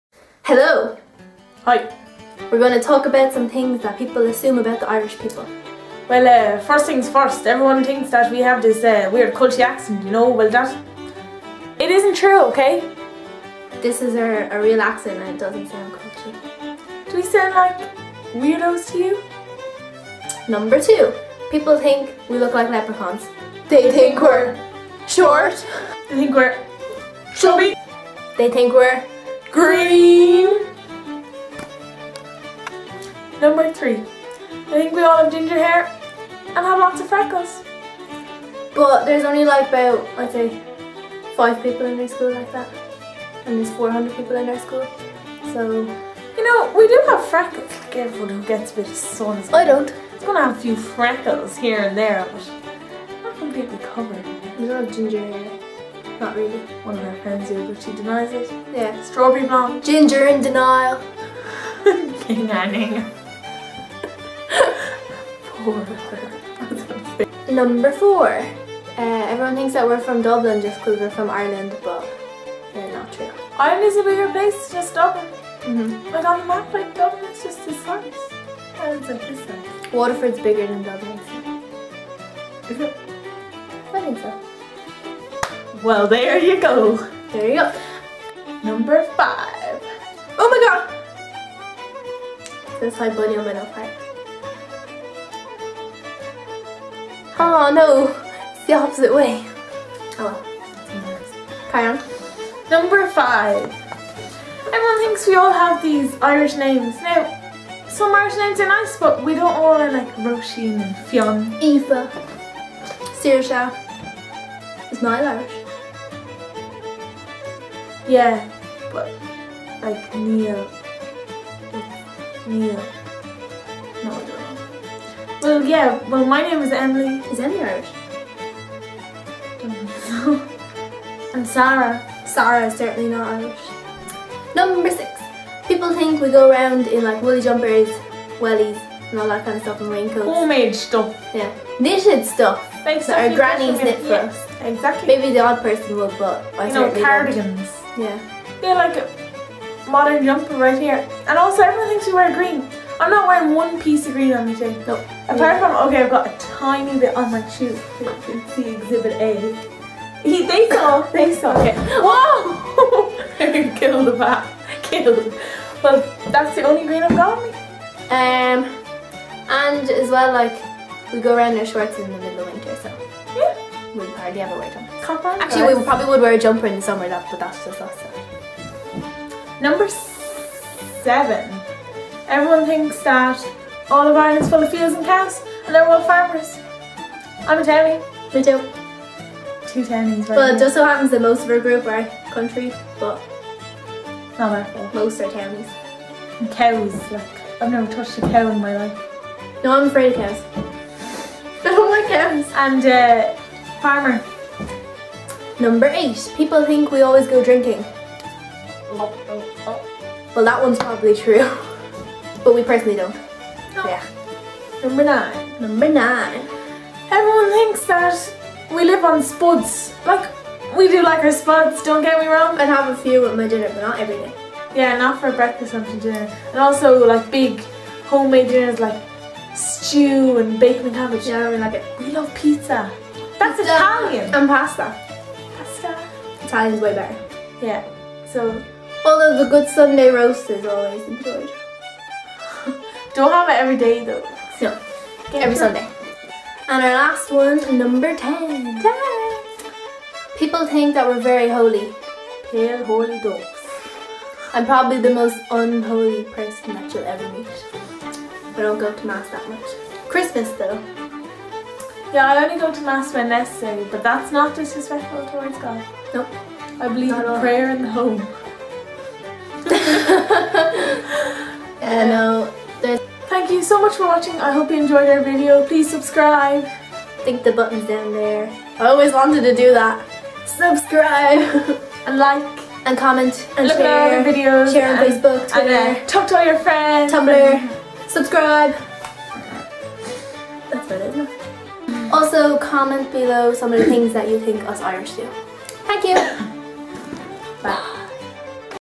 10 things people assume about the irish
Me and Emily made a video of 10 ten things people assume about the Irish